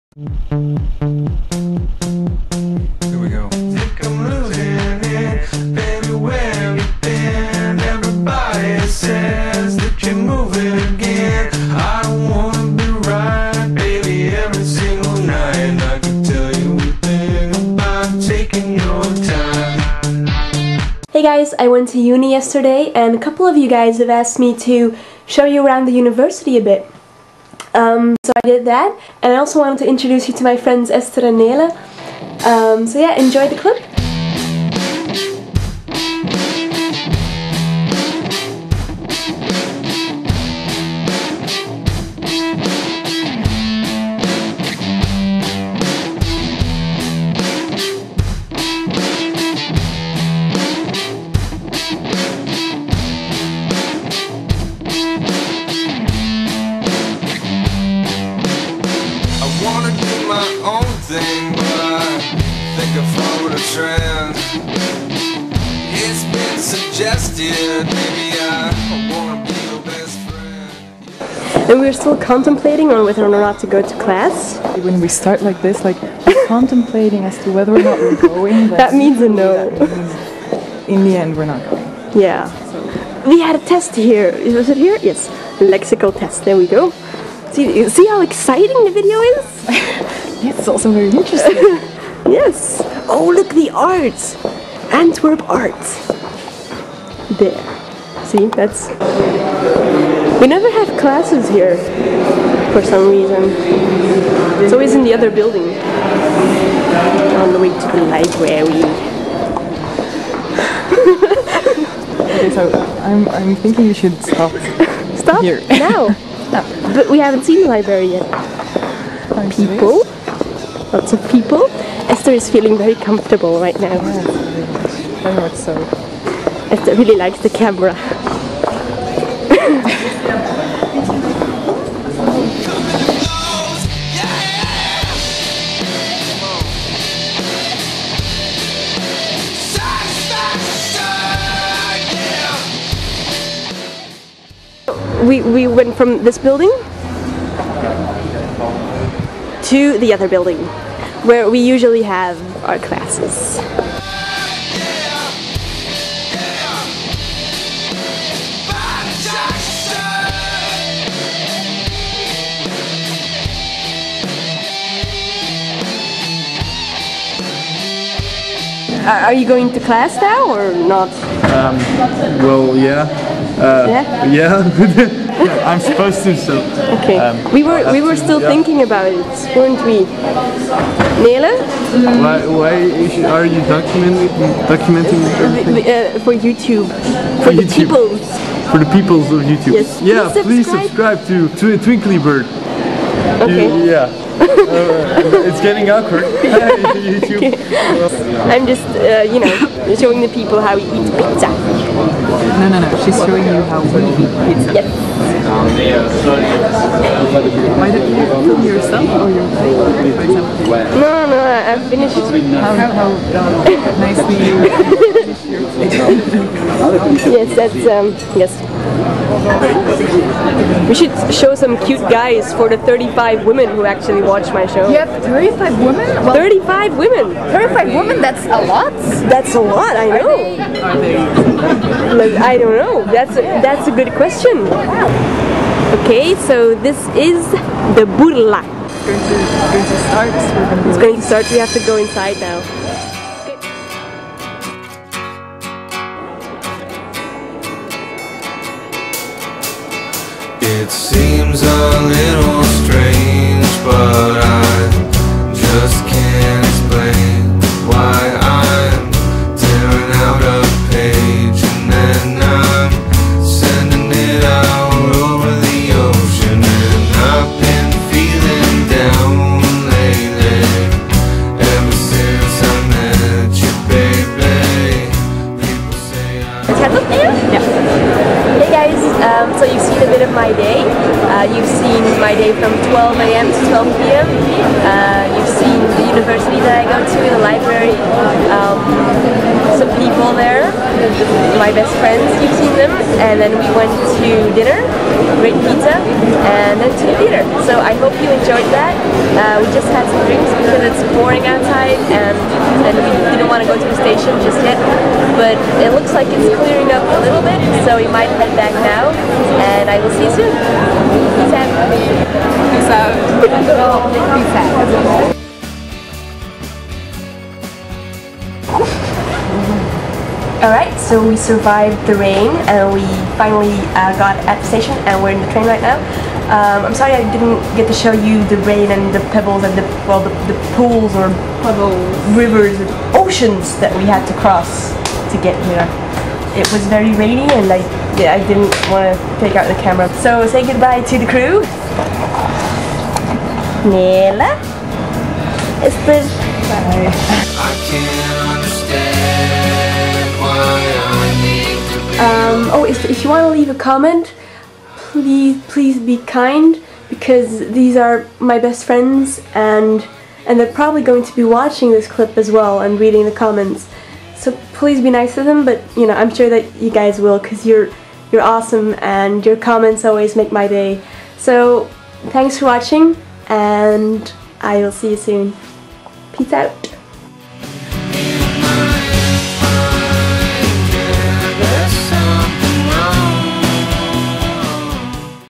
A Day at Uni
Some of you had asked to film the university I go to, so I decided to take you guys along for the day!